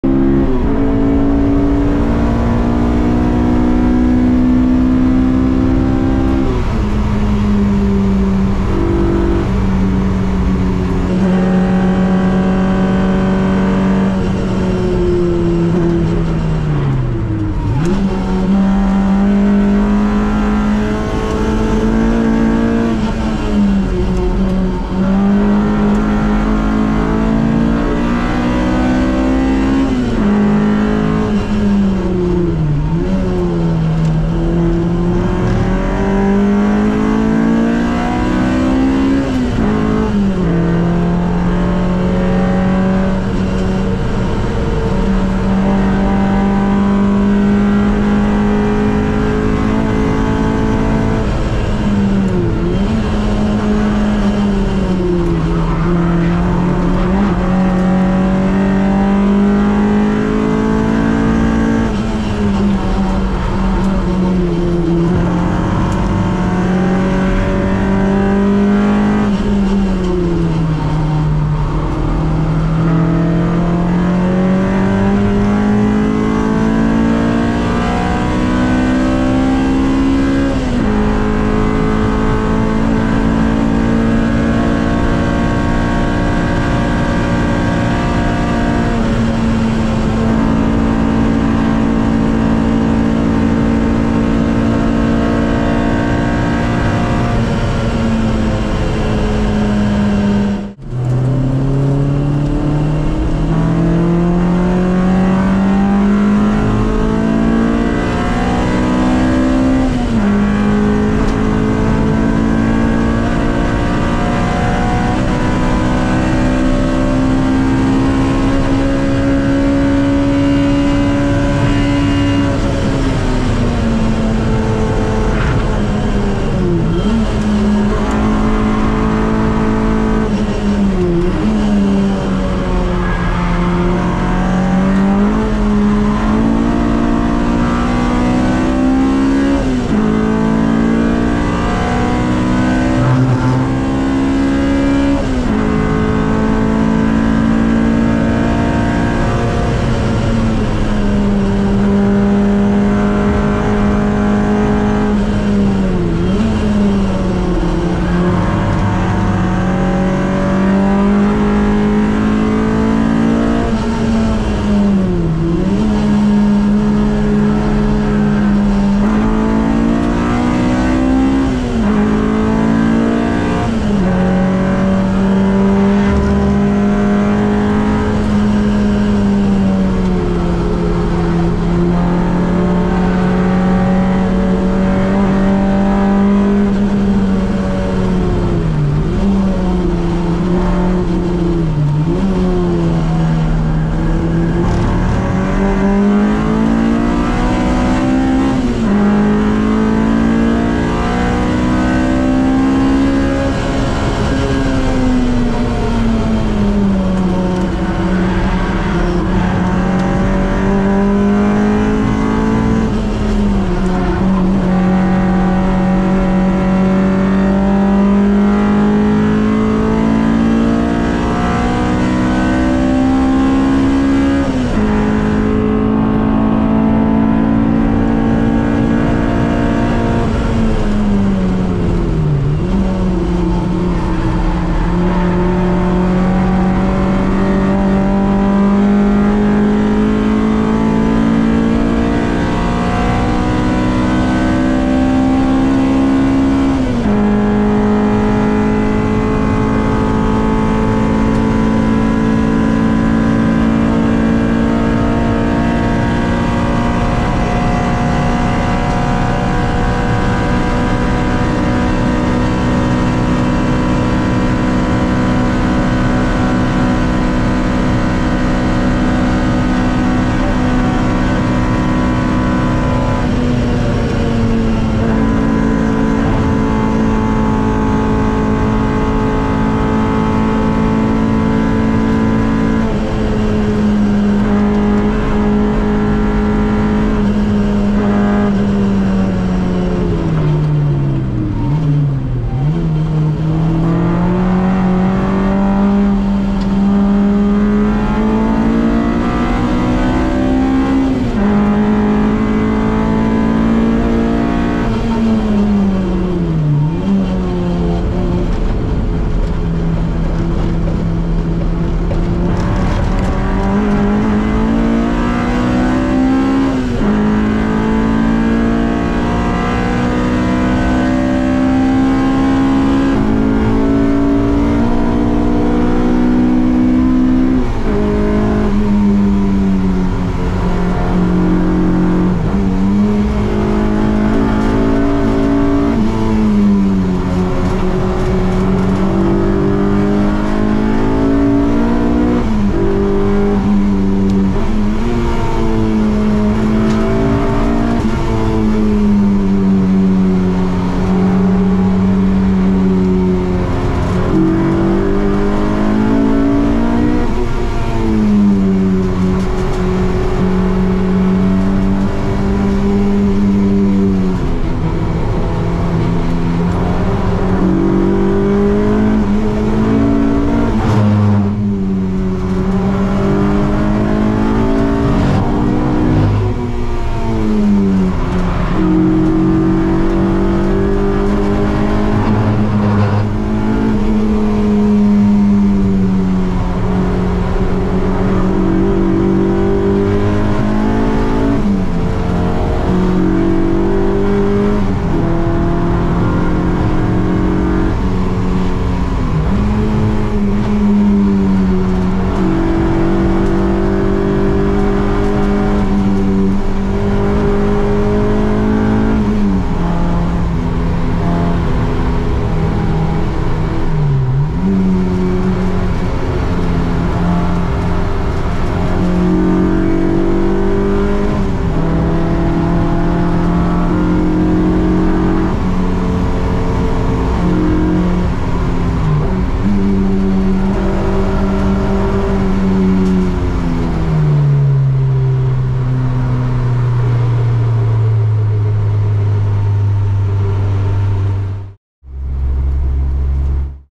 Integra Type R K24 Vs Civic EF B18C / Nürburgring Battles // TCT-Performance
It was time to improve the lap with @kayhesse and post it another time to share this banger again! / Thanks for the fun lap Kay and all the support during the build.